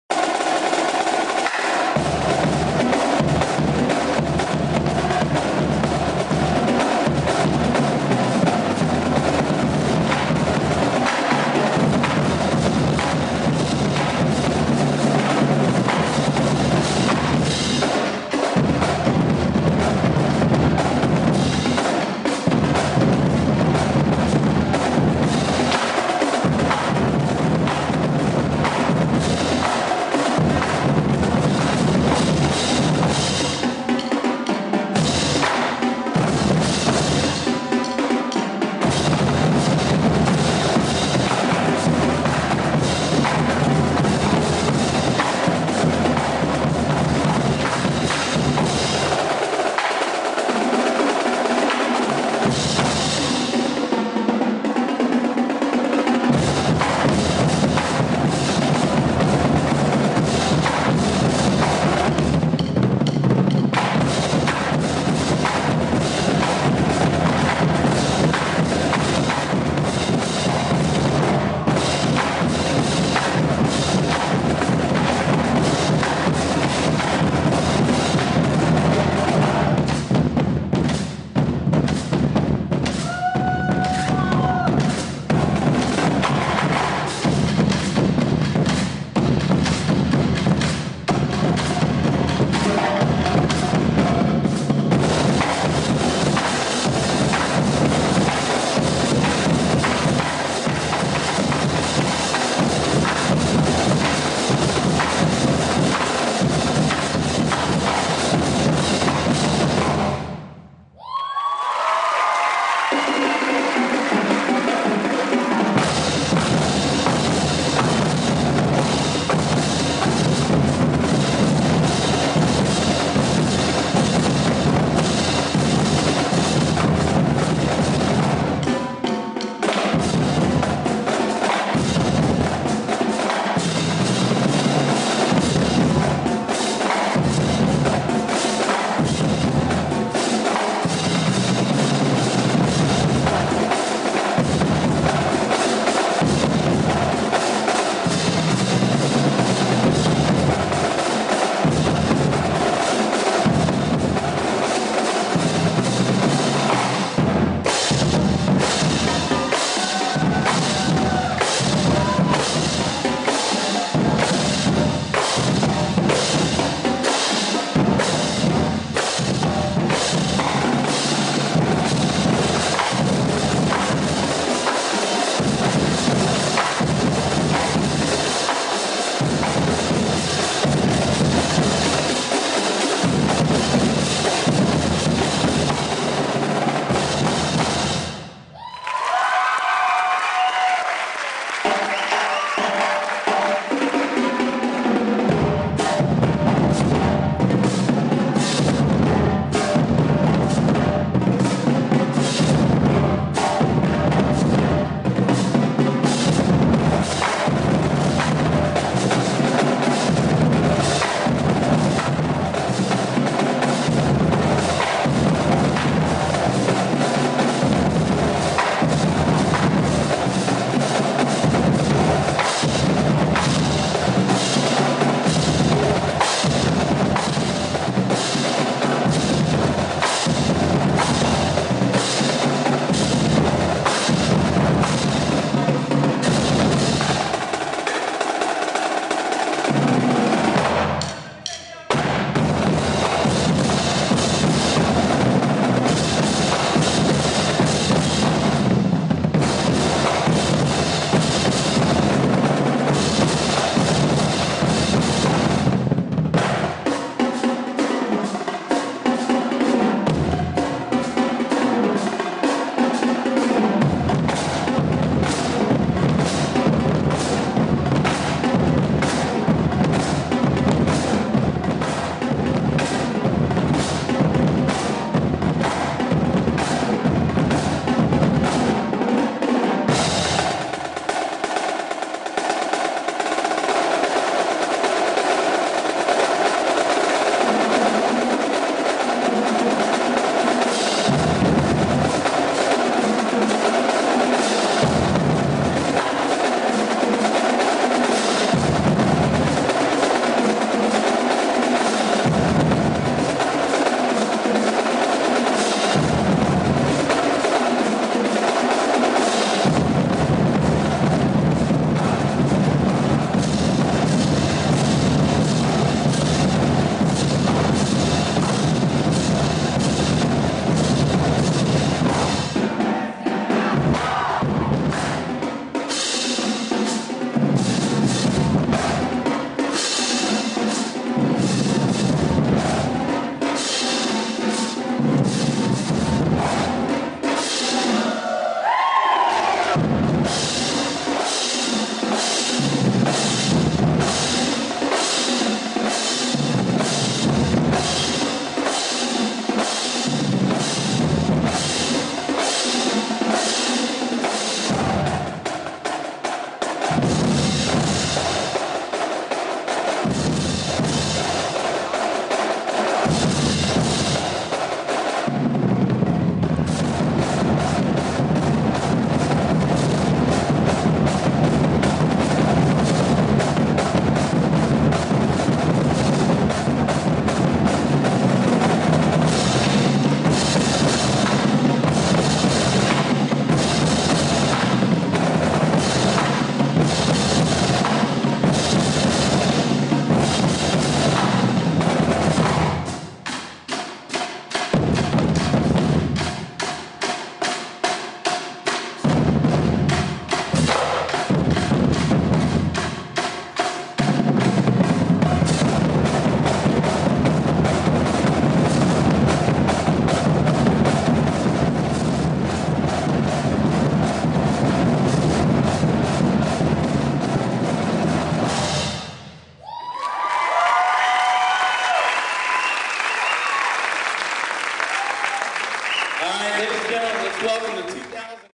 Drumline 2008 Seminole High School Sanford FL
Seminole High School's Drumline at the Fall Concert 2008.  In Sanford, FL. We are missing one of our tenors...